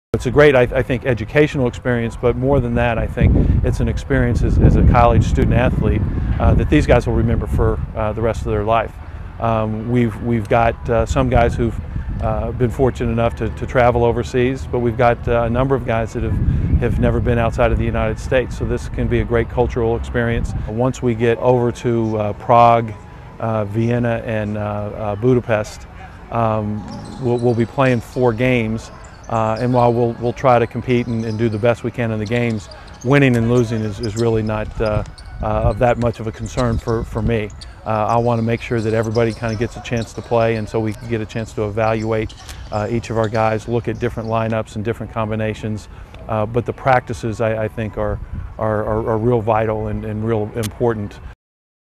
2011 European Tour Intro - American University Eagles
Hear Head Coach Jeff Jones' comments prior to the team's 10-day European Tour: AUeagles.com